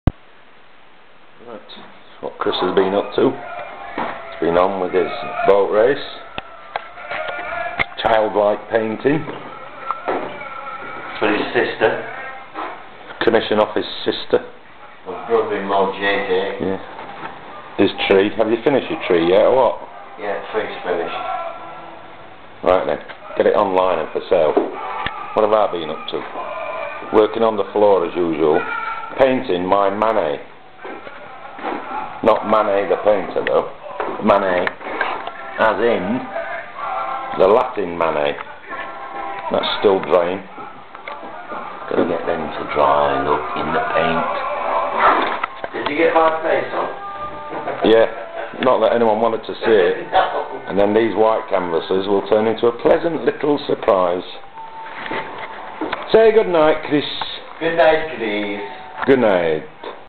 Painting a manet & painting in a childlike style
I'm not painting in the style of manet the artist,but doing a painting with the latin word manet in it's title. Chris is trying to do a childlike painting style,which picasso said was the most difficult technique to capture !!
14th august 2009